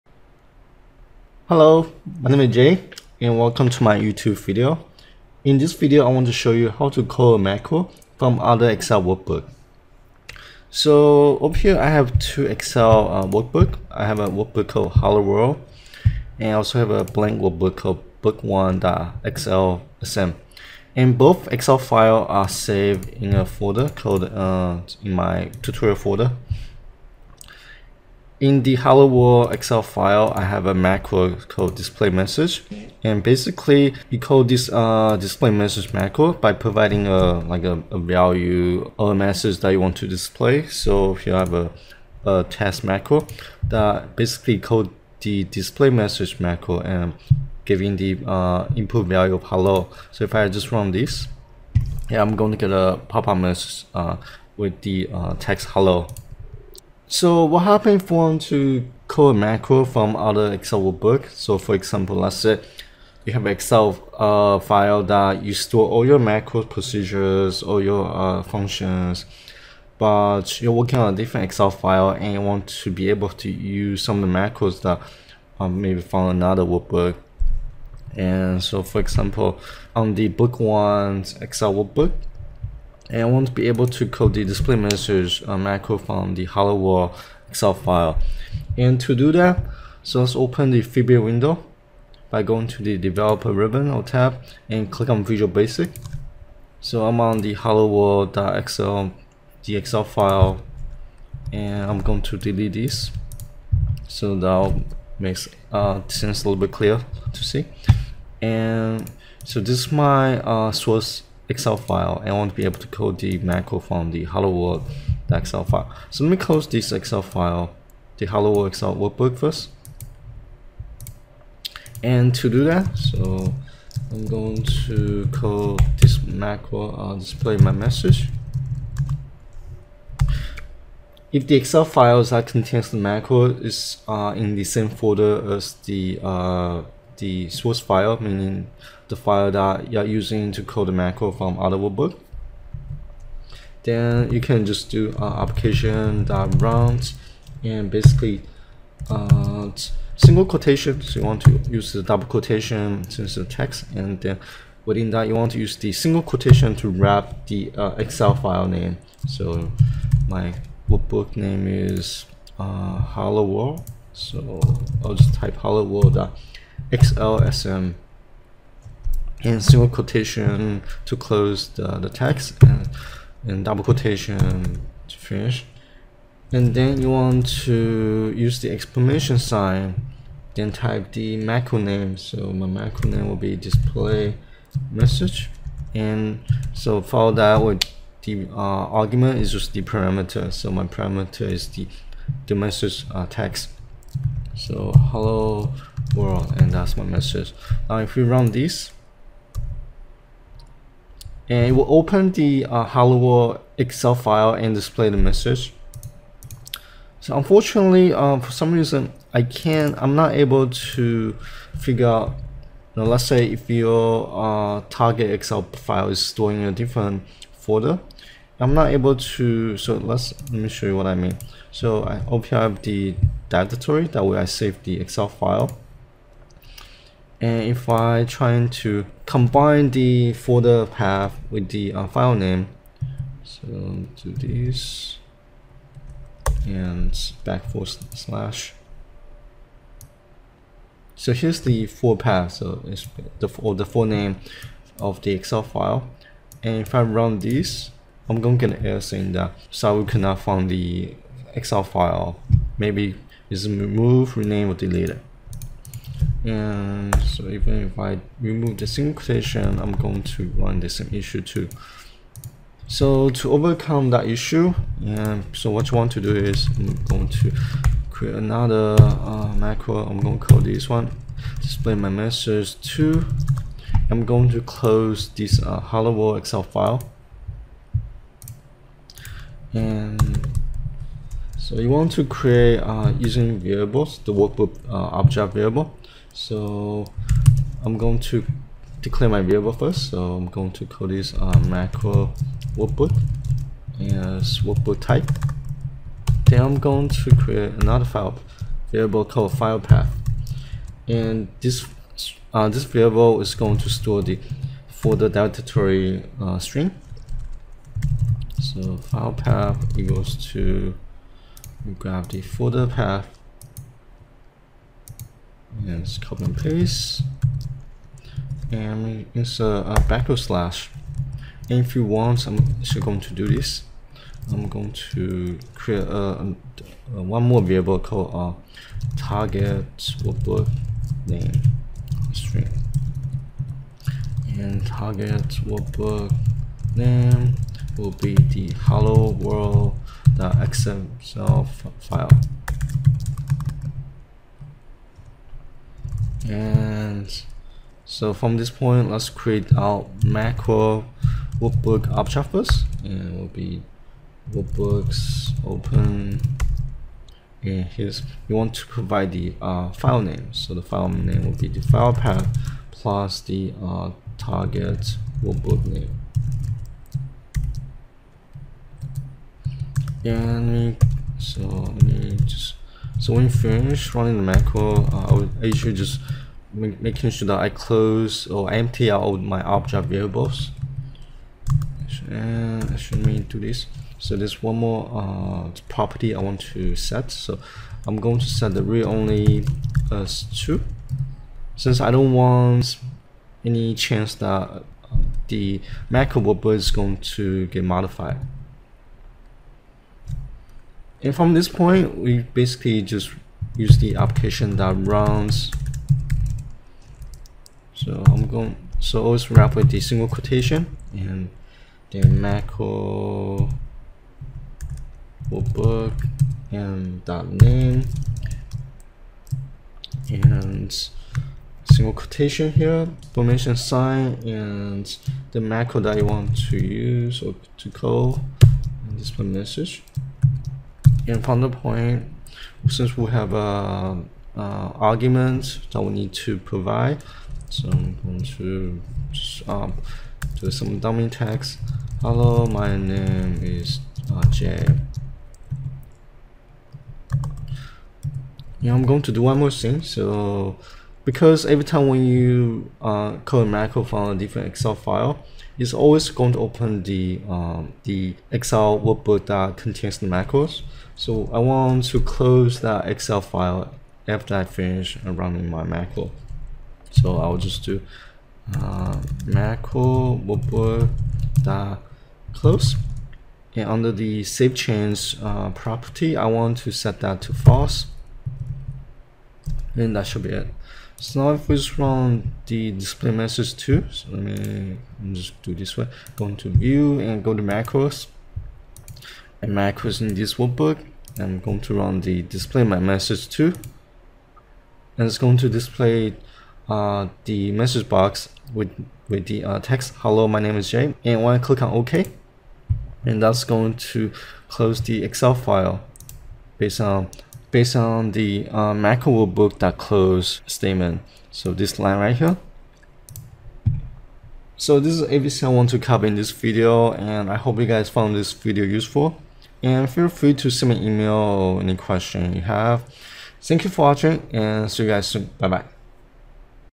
How to call a Macro from another Excel workbook | MS Excel VBA Tutorial
Sometimes you may want to run a macro from another Excel workbook. Or maybe you have an Excel workbook which you store all your macros and you want to use some of those macros without redoing the same work.

In this video I will show you how to call a macro from a other Excel workbook.